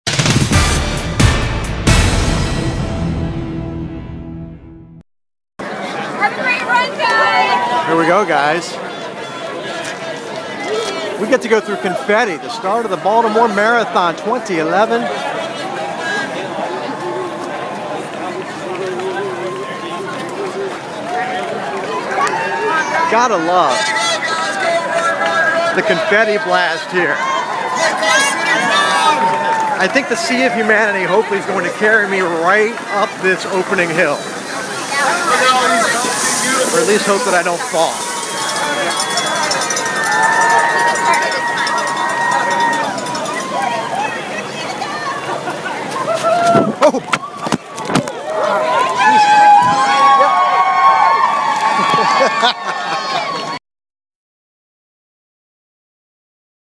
Pete Gilbert at the Start of His Baltimore Marathon Relay
WBAL's Pete Gilbert ran a leg of the relay during today's Baltimore Marathon to benefit the Cool Kids Campaign